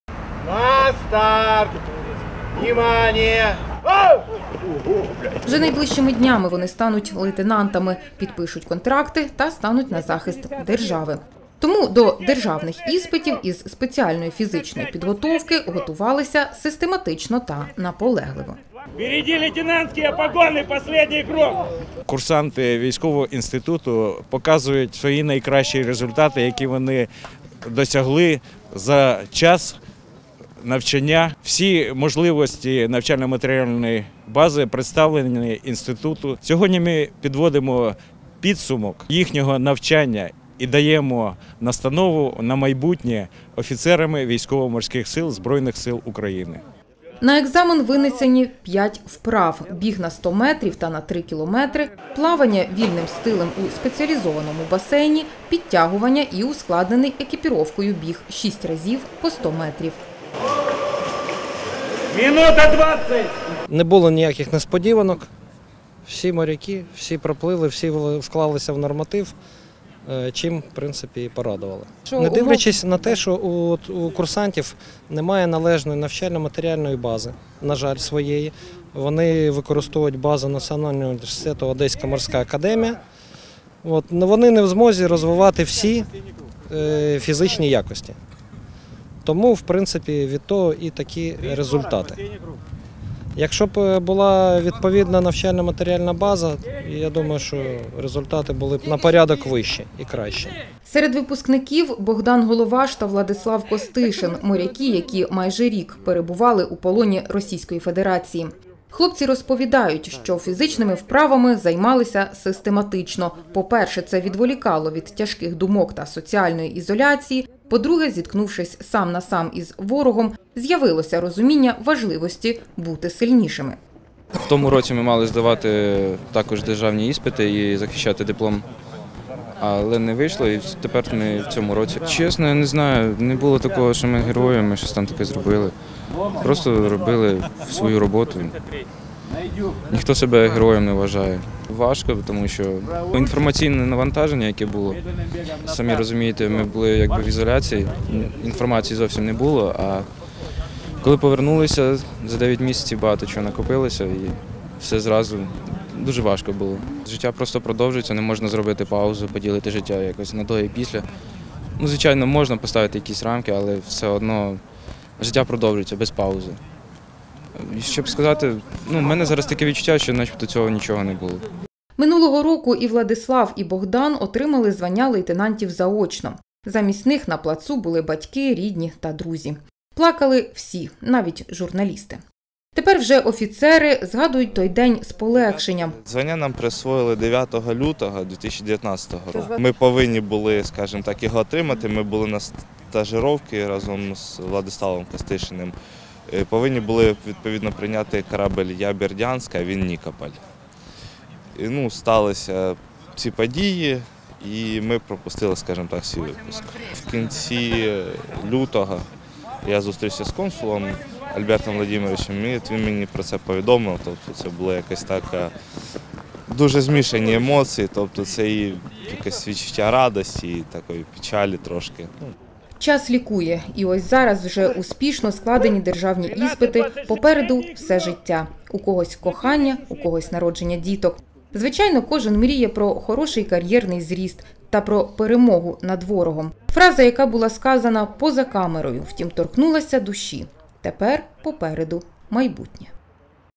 Курсанти інституту ВМС НУ "ОМА" склали державні іспити зі спеціальної фізичної підготовки.
Тому до державних іспитів із спеціальної фізичної підготовки готувалися систематично та наполегливо.
На екзамен винесені 5 вправ: біг на сто метрів та на 3 км, плавання вільним стилем у спеціалізованому басейні, підтягування і ускладнений екіпіровкою біг 6 разів по сто метрів.
Серед випускників Богдан Головаш та Владислав Костишин, моряки, які майже рік перебували у полоні Російської Федерації. Хлопці розповідають, що фізичними вправами займалися систематично, по-перше, це відволікало від тяжких думок та соціальної ізоляції, по-друге, зіткнувшись сам на сам із ворогом, з’явилося розуміння важливості бути сильнішими.
Минулого року і Владислав, і Богдан отримали звання лейтенантів заочно. Замість них на плацу були батьки, рідні, друзі. Плакали всі, навіть журналісти. Тепер вже офіцери згадують той день з полегшенням. Про вручення погонів їм розповів консул: радість змішалася з гіркотою. Тепер – це наче страшний сон, який нарешті закінчився.
Час лікує. І ось зараз вже успішно складені державні іспити, попереду все життя: у когось кохання, у когось народження діток. Звичайно, кожен мріє про хороший кар’єрний  зріст та про перемогу над ворогом. Фраза, яка була сказана поза камерою, втім, торкнулася душі: «Тепер попереду майбутнє».